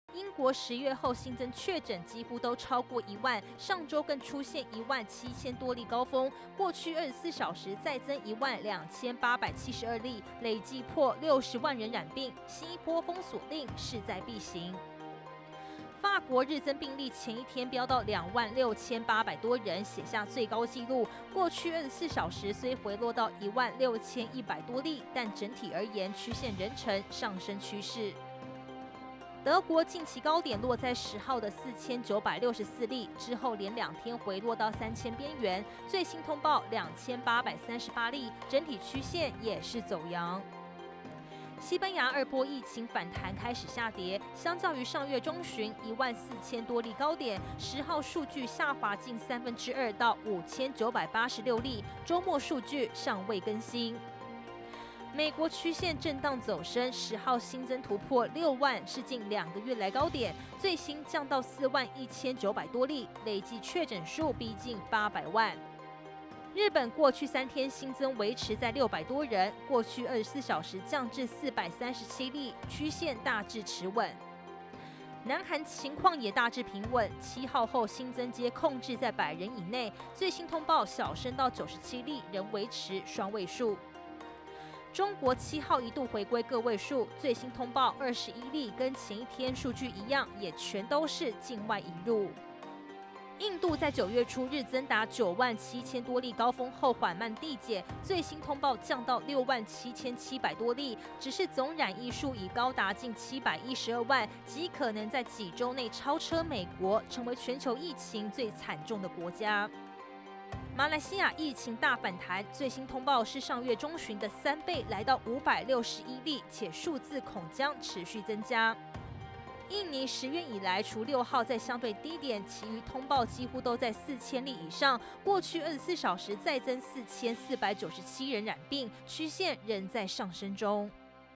英法德二次疫情難控 美國累計確診逼近8百萬 南韓新增維持百人內｜寰宇新聞20201012
截至台灣時間12號中午，新冠肺炎總確診已經超過3774萬人，病故數破100萬。亞洲方面，印度總病例數，極可能在未來幾周，成為全球最多的國家。美國疫情則有小幅攀升趨勢。另外在歐洲，英法德二波疫情仍尚未壓制住。